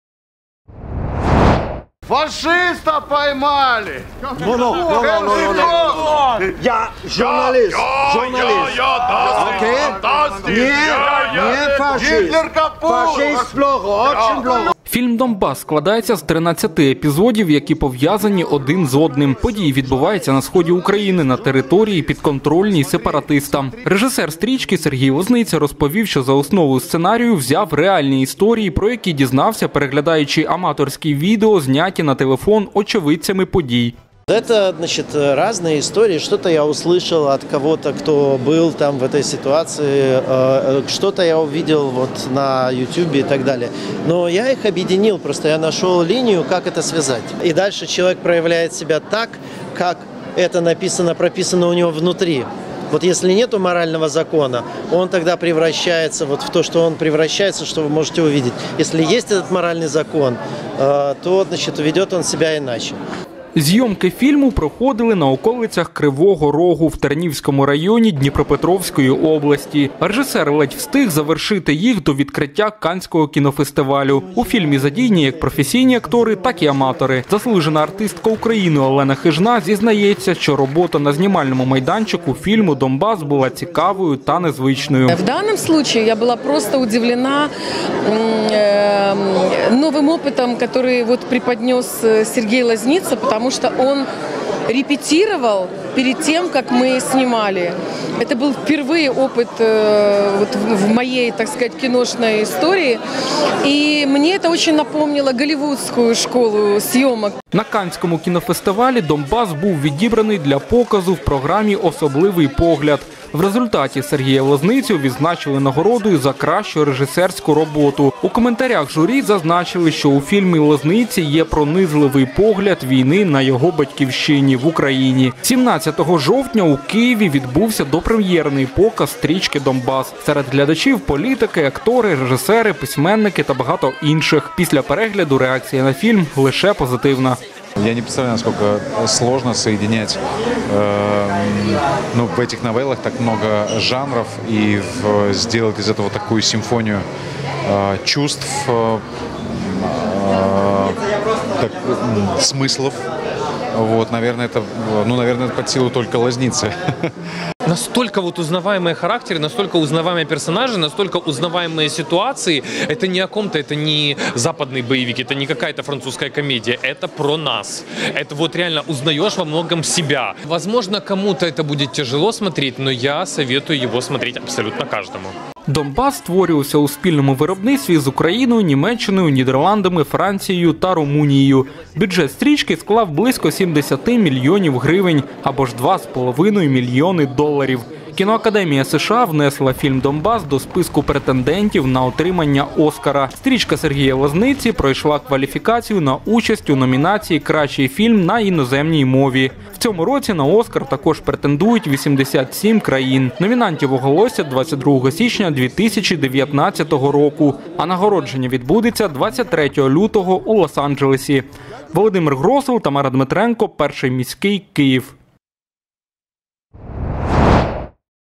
«Донбас» в українському прокаті
Національний кандидат на премію «Оскар». Фільм «Донбас» виходить в український прокат. Це драма, де усі події відбуваються на сході України, на території, підконтрольній сепаратистам. Стрічку ще в травні цього року високо оцінили на Канському міжнародному кінофестивалі. З 18 жовтня фільм виходить в український прокат. На допрем’єрному показі побували наші столичні кореспонденти.